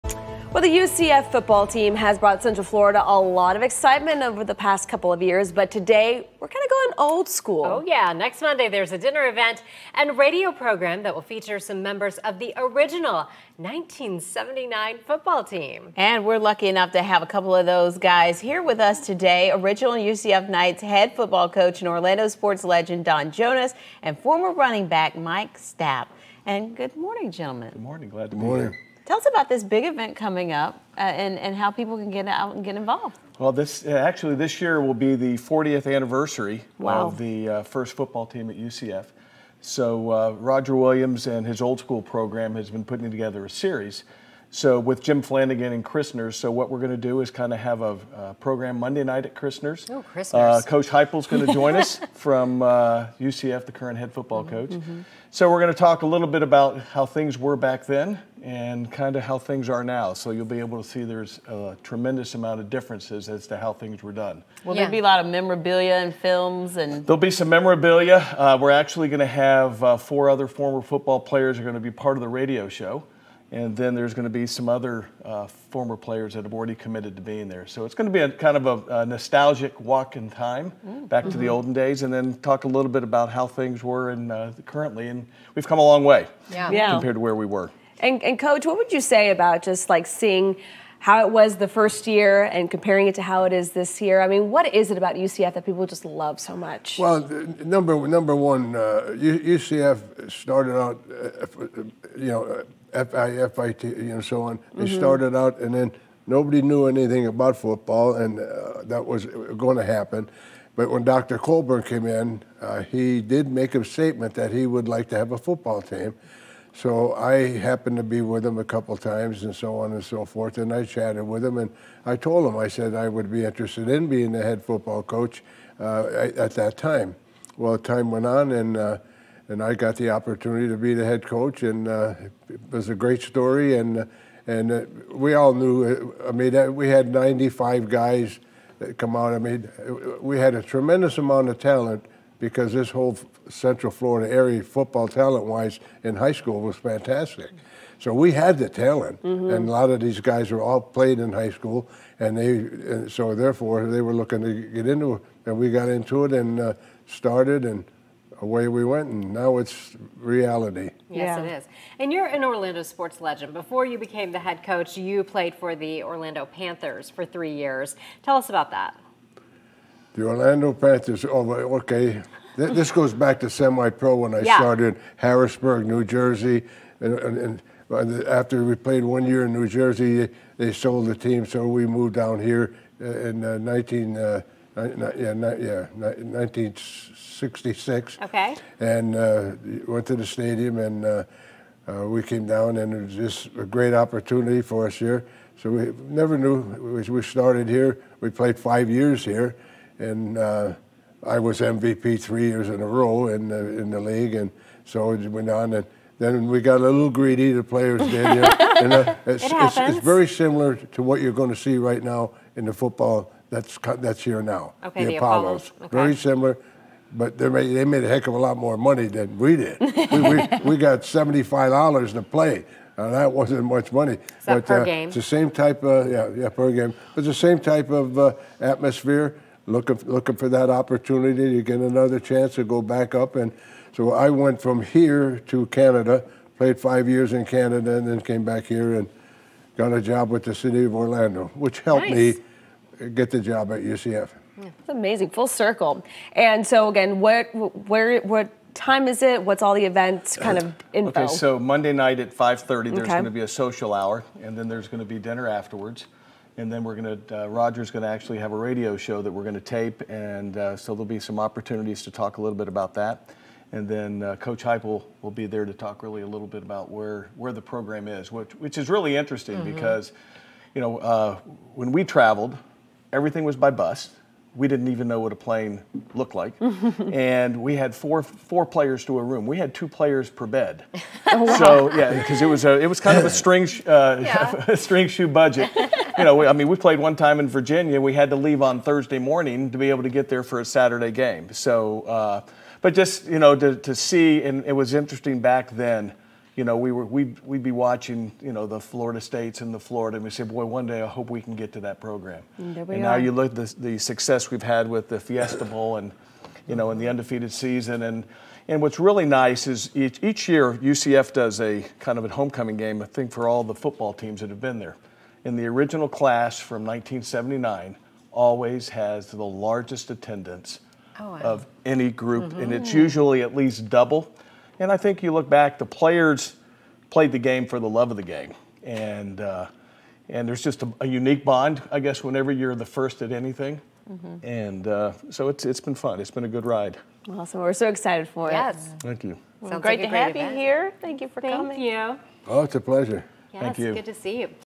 News 6 talks to members of the original 1979 UCF football team
This year marks 40 years since the first UCF football team. Original Knights Head Coach Don Jonas and former Knights running back, Mike Stapp talk to News 6 about an upcoming dinner and radio show where they will talk about UCF football then and now.